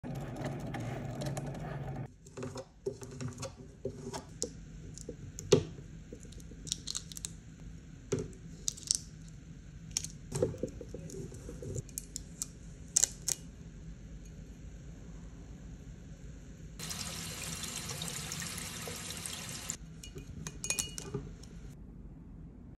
How to etch glass with Cricut! 😎
Try this quick and easy craft! Using Armor Edge etching cream, you can etch a design onto a glass surface within only a couple of minutes! Perfect craft for beginners and much easier than you think…